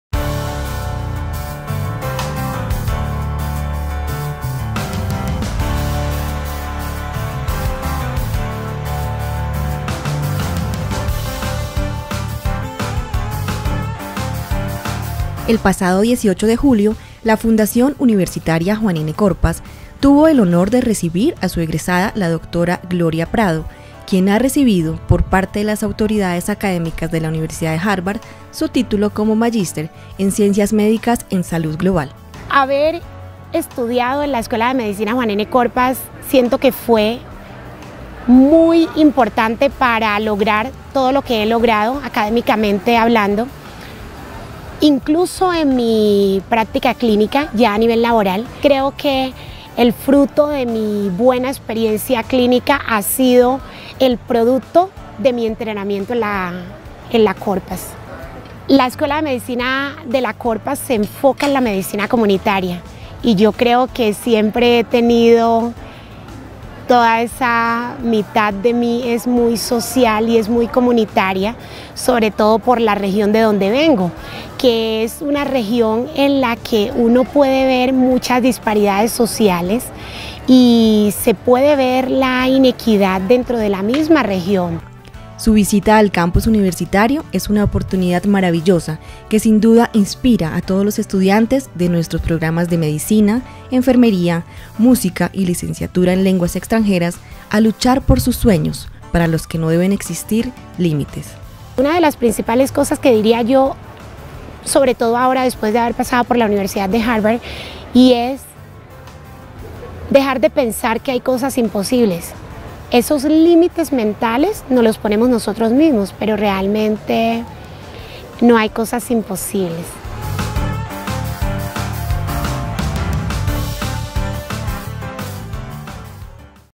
Dra. Prado graduada en Harvard: motivo de orgullo para la Corpas
El pasado 18 de julio la Fundación Universitaria Juan N. Corpas, tuvo el honor de recibir a su egresada la Dra. Gloria Prado, quien ha recibido por parte de las autoridades académicas de la Universidad de Harvard su título como Magister en Ciencias Médicas en Salud Global. ¡Conócela!